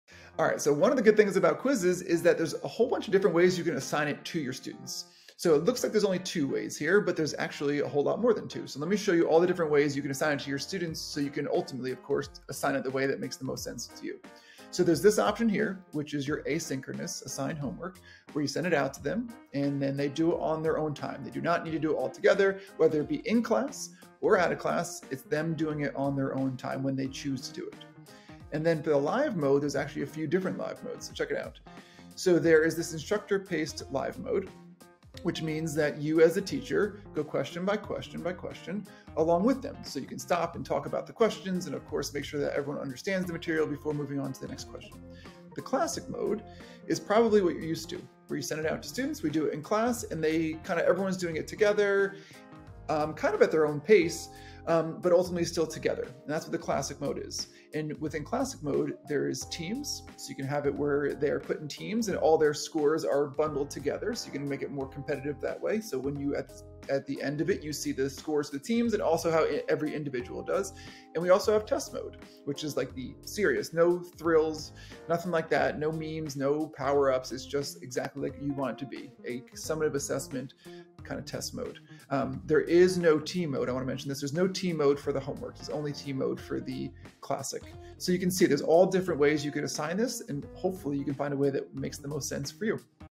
The different modes in Wayground (formerly Quizizz)!
Quizizz has a number of modes that you can use for assigning quizzes and lessons to your students.  In this video, Kyle Niemis shows you just how many modes are on Quizizz that you can use to assign quizzes for formative and summative assessments, and instructional lessons prescribed in your curriculum to your students.

Deliver instruction that's relevant for every student, now with a boost from AI. 💜